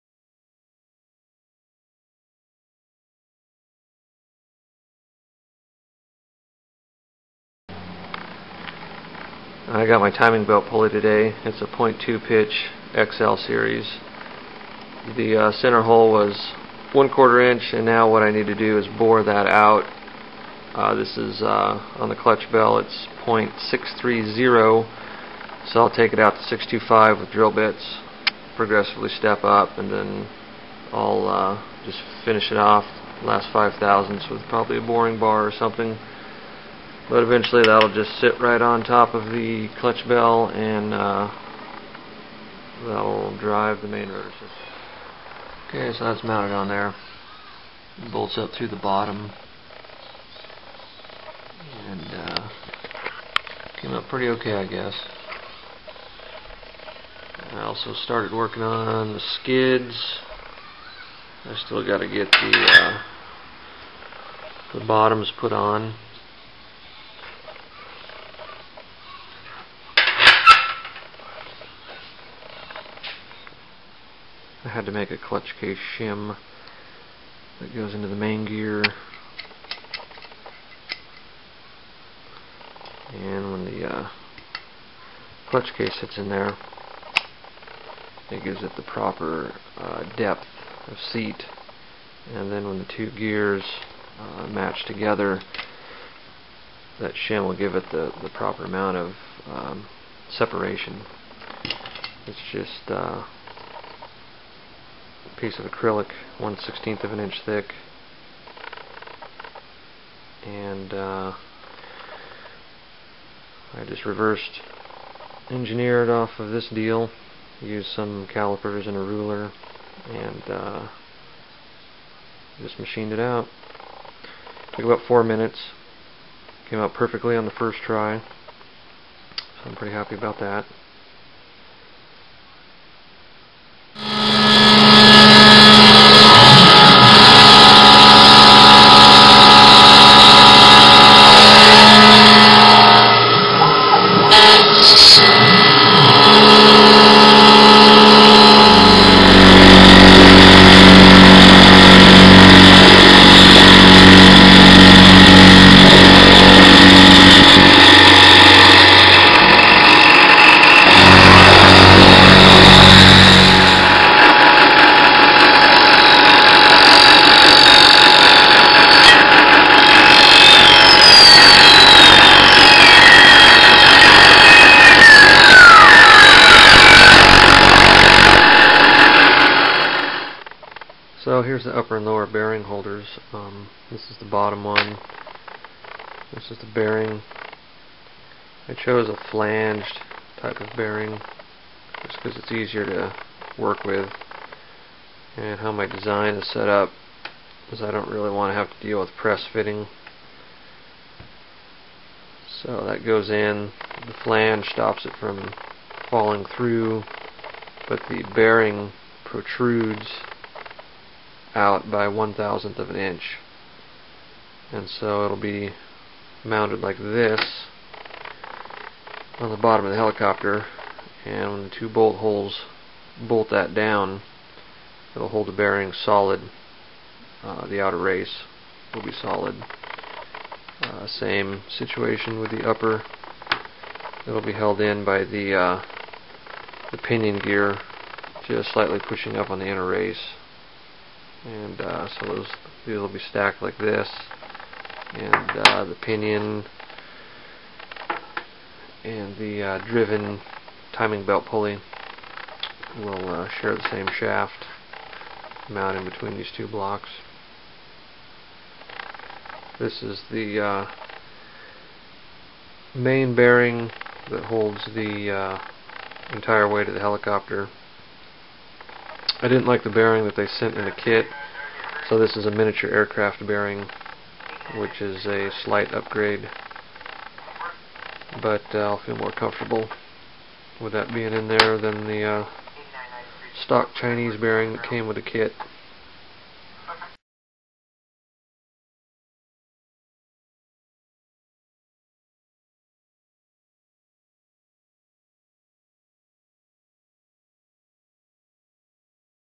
HK600GT Gasser Conversion part 4
Part 4 of a HK600GT gasser conversion. Drive train part production.